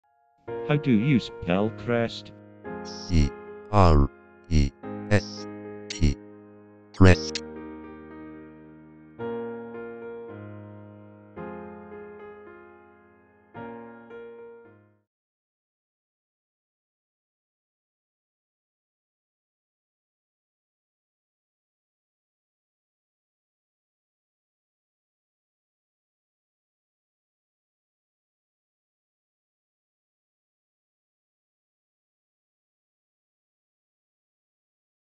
crest spelling
Learn how to spell and pronounce crest.